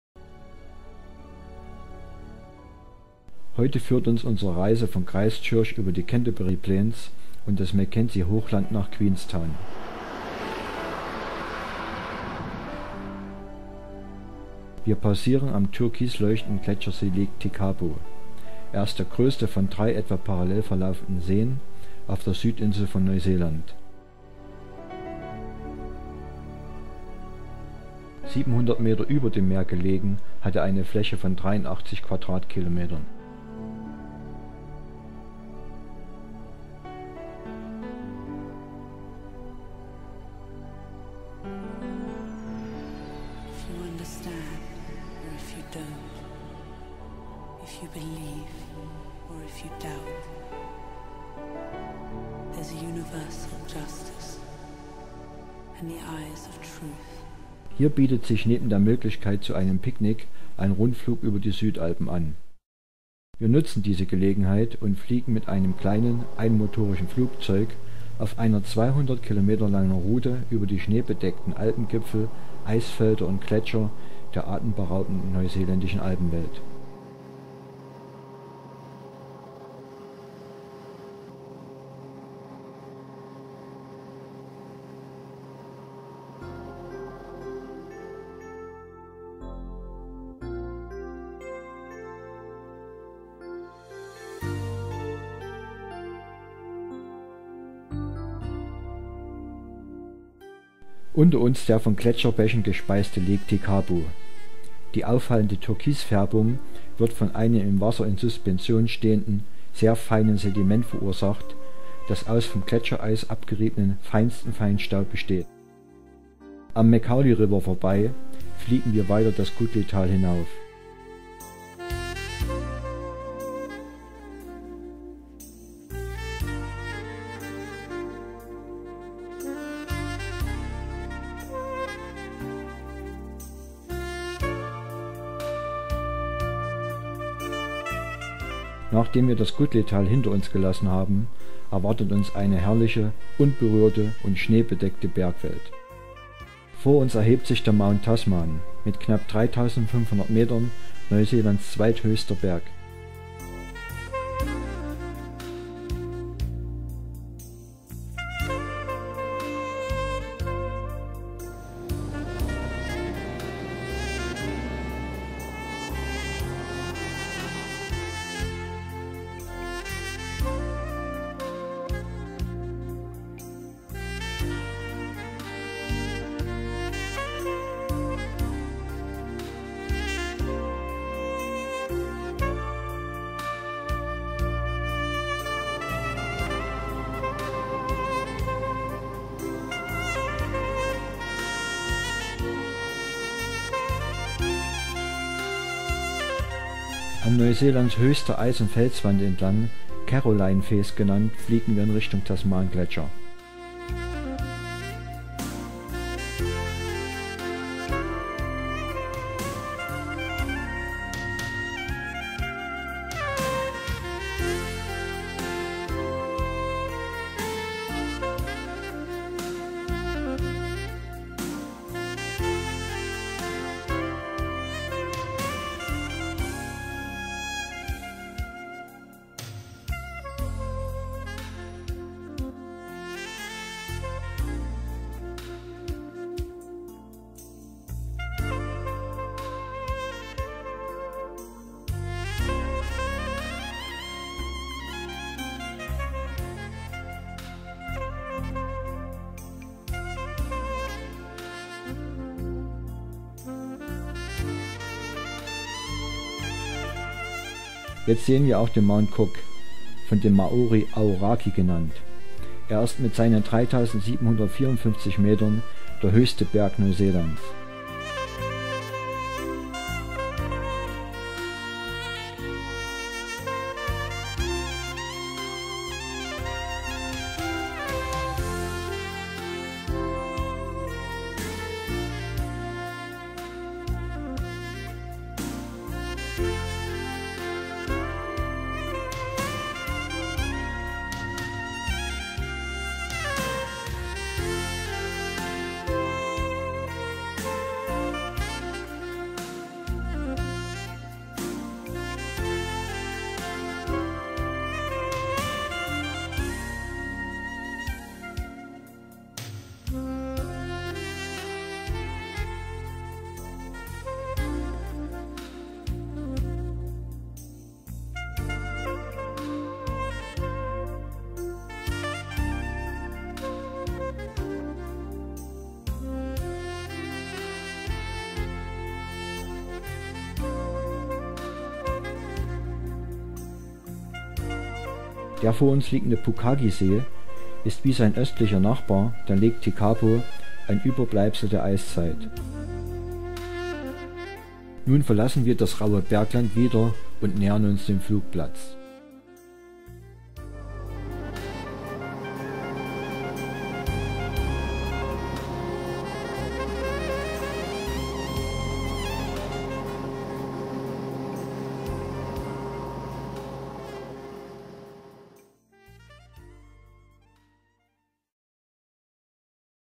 NZ 02/26 Neuseeland - Paradies am Ende der Welt
Die neuseeländischen Südalpen aus der Vogelperspektive.
Unterwegs auf der Südinsel

Teil 2 einer aus 26 Teilen bestehenden Dokumentation einer 3 wöchigen Neuseelandreise.
Den Reisebericht und viele tolle Fotos zu diesem und vielen anderen schönen Reiseabenteuern findest Du